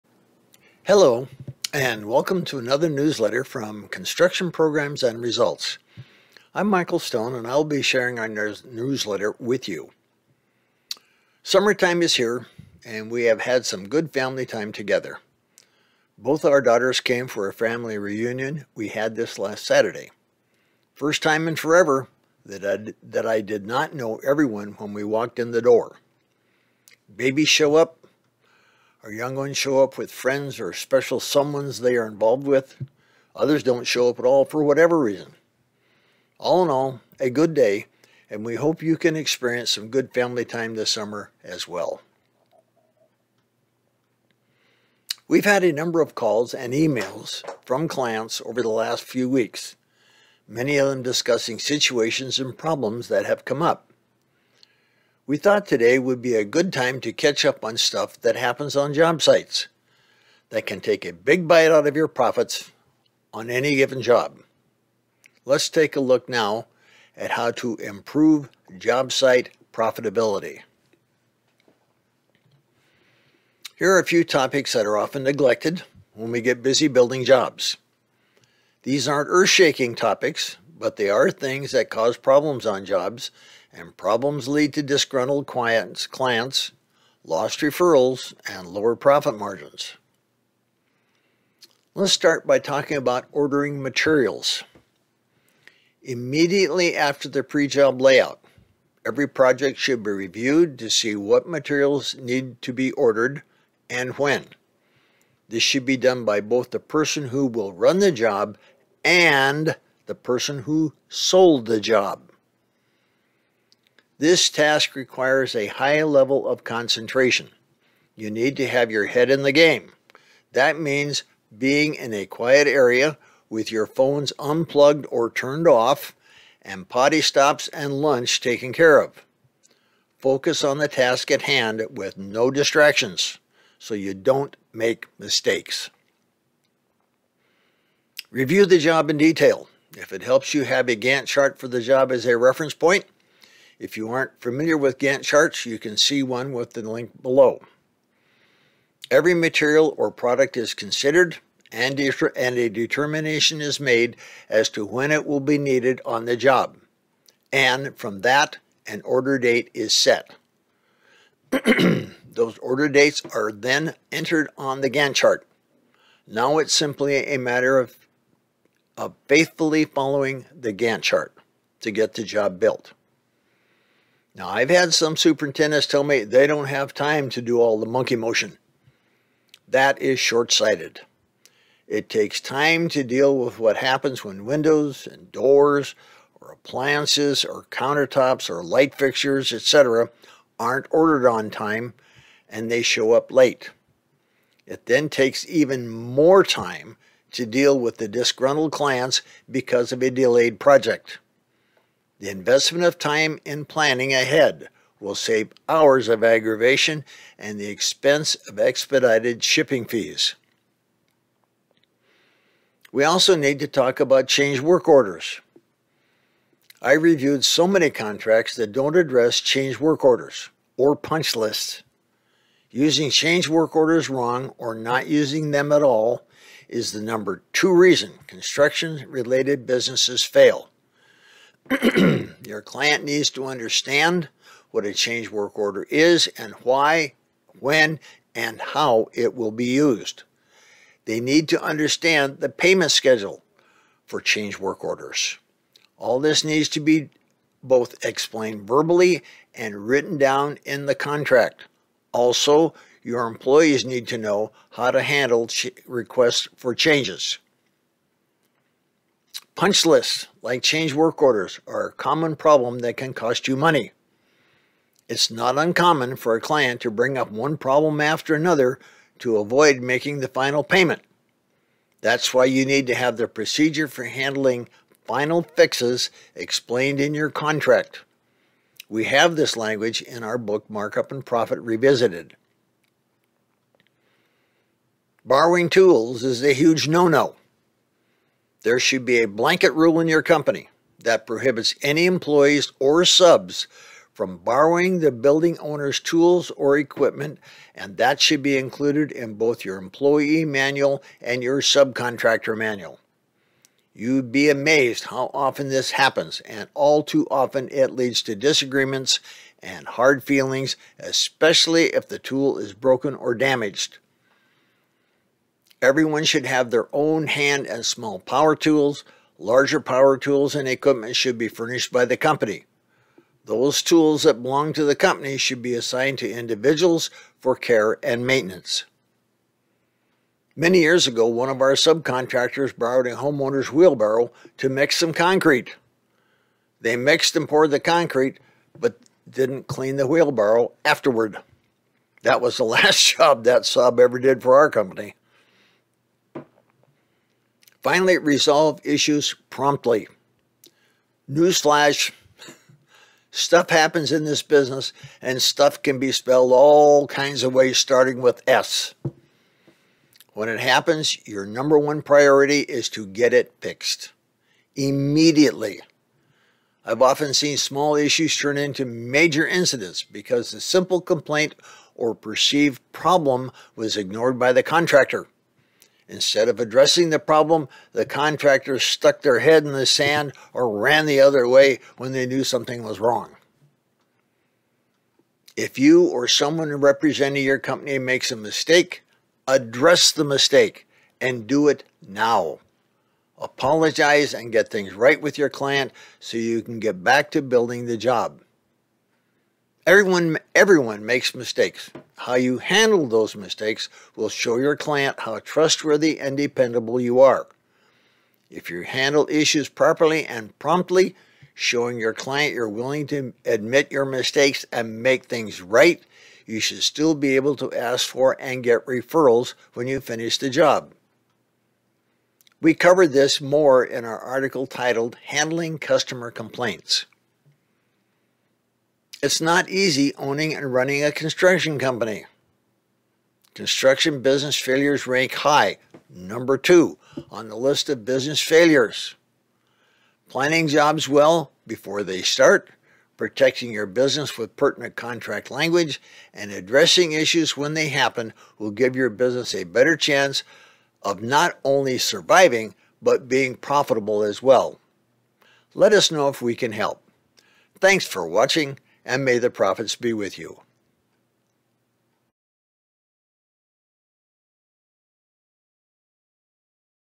Construction Jobsite Productivity
Michael talks about issues that impact the efficiency of your projects. That matters because time wasted on a job comes right out of your bottom line.